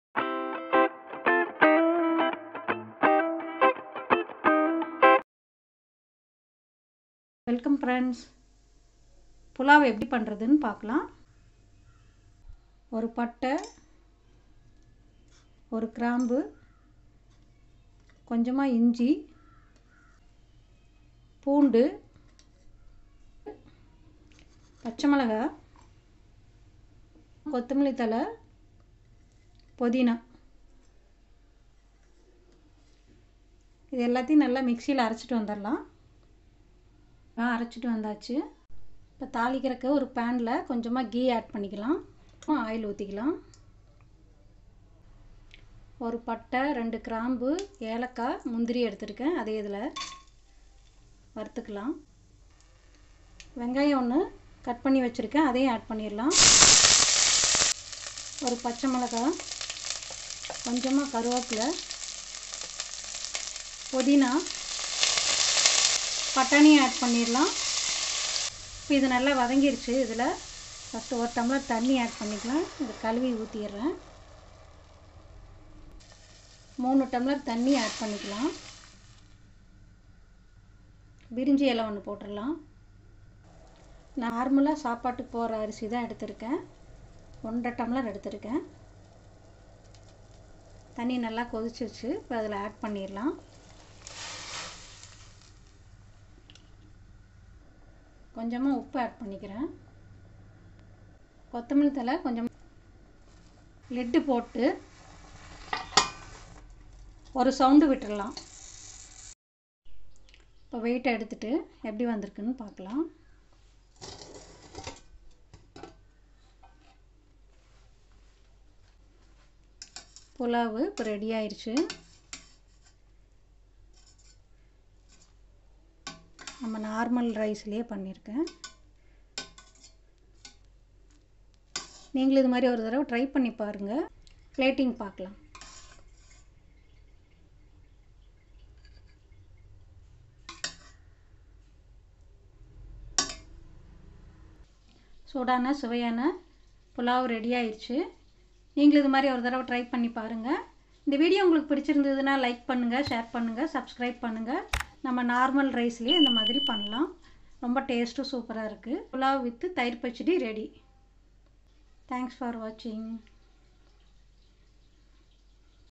Green Peas Pulao || How to prepare Green Peas Pulao?Quick & Only Normal Rice Recipe | பட்டாணி சாதம்
Shanthi Baskaran
Ingredients
Onion
Ginger, Garlic
Green Chilli
Cinnamon
Cloves
Cardamom
Cashew
Bay leaves
Pudina
Curry leaves
Coriander leaves

Hi Friends,

For more Healthy & Tasty Recipes
Veg & Non Veg Recipes

Video Click Link